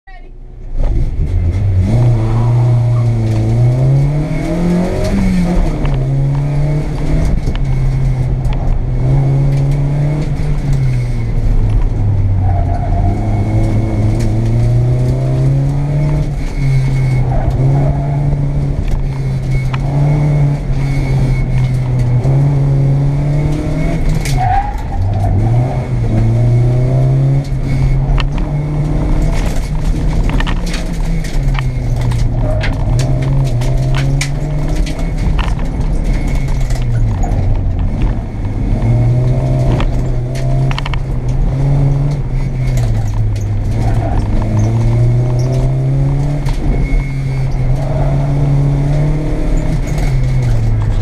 NCCAutocross Points Event #5 - 8/14/16 - Bowie Baysox Stadium
1986 VW Scirocco